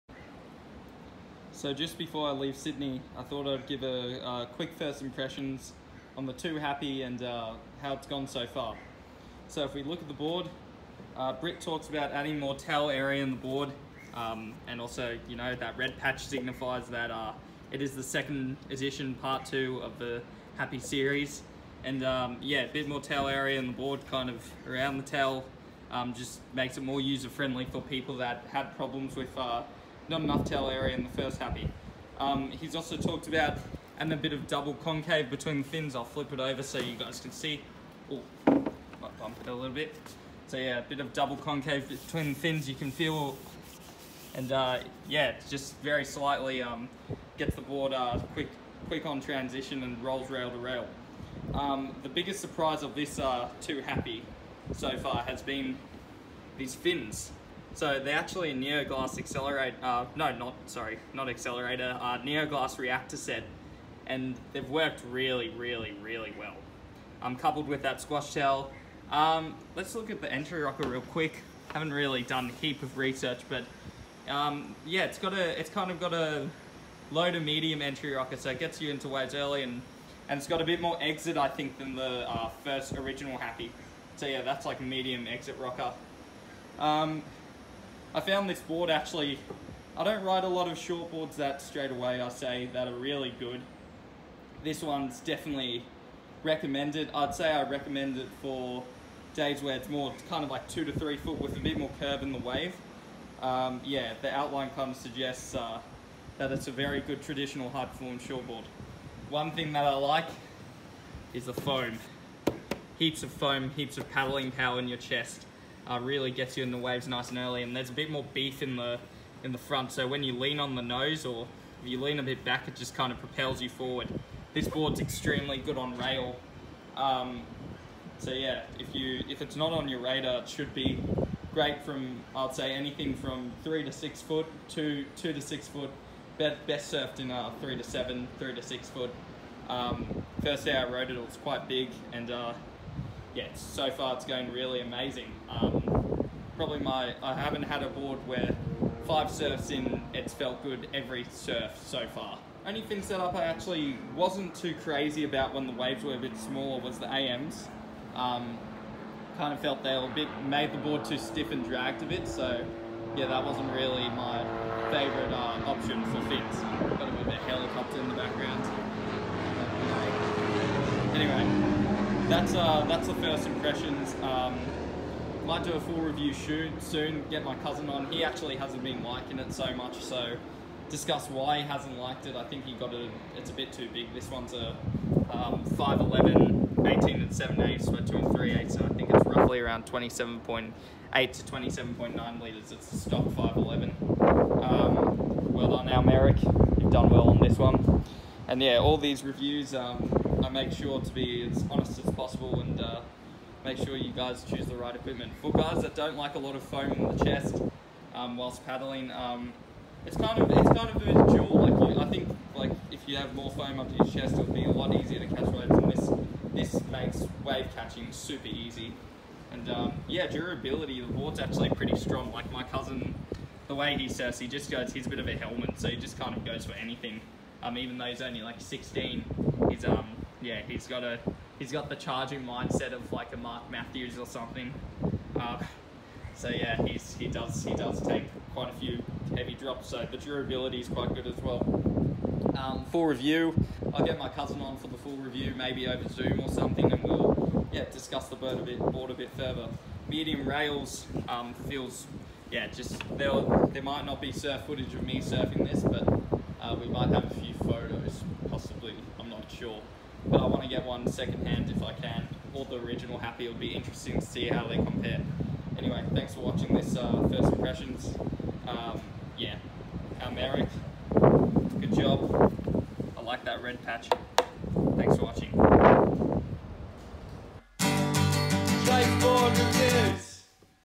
Channel Islands Two Happy (EP.6 First Impressions A Great Performance Shortboard)
Hey fellow surf addicts, today I'll be giving my First Impressions on the Two Happy by Channel Islands. I was riding this down in Sydney on a visit to see family and friends, and really fell in love with it. I'm going to hassle my cousin to do a board swap or try buy it off him, as he's not as stoked with it as I am.

Fun Fact: Stab In The Dark starring Taj Burrow, this board came runner up (2nd place), losing to the Inferno 72.

Conditions: 2-7 ft (Best in 3-7 ft)

Fins so far:

- FCS 2 Reactors (Medium)
- FCS 2 Am's (Medium)